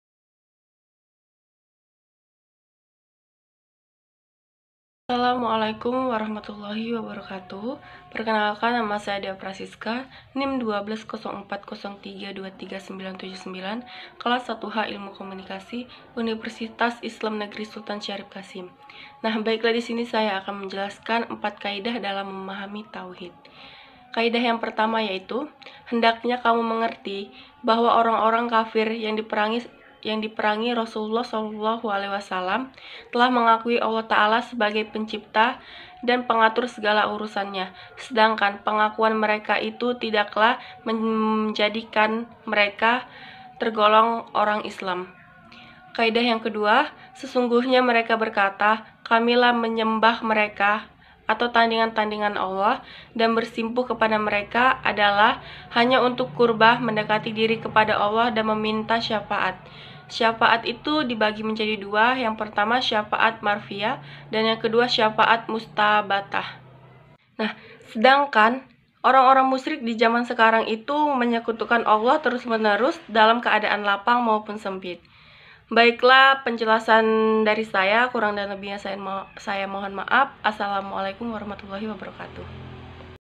KULTUM: KAIDAH UTAMA MEMAHAMI TAUHID
Sebagai seorang muslim kita wajib memahami konsep tauhid dan syirik dalam agama kita. Konsep ini telah dijelaskan dengan mudah oleh Syaikh Muhammad bin Abdulwahhab dalam kitabnya Qowaidul Arba’.

DEA PRASISKA